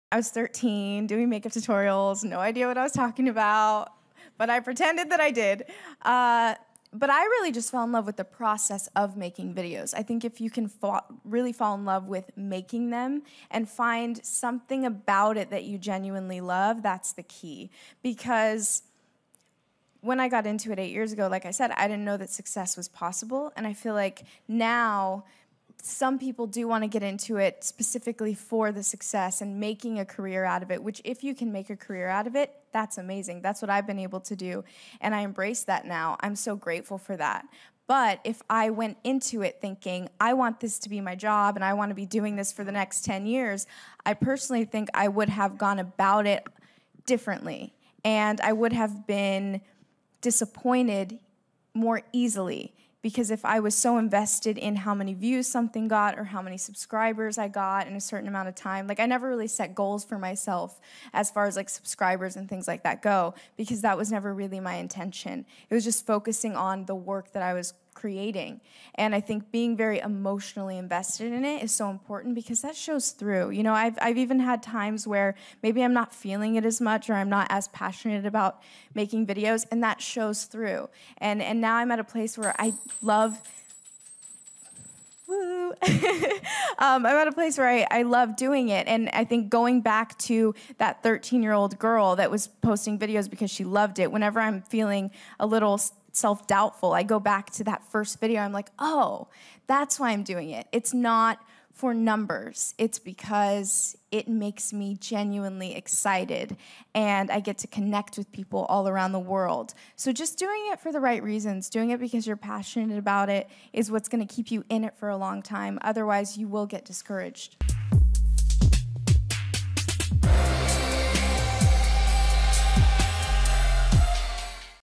Bethany Mota's Advice For Aspiring YouTubers
Vlogger and author Bethany Mota dishes on how to start a YouTube channel.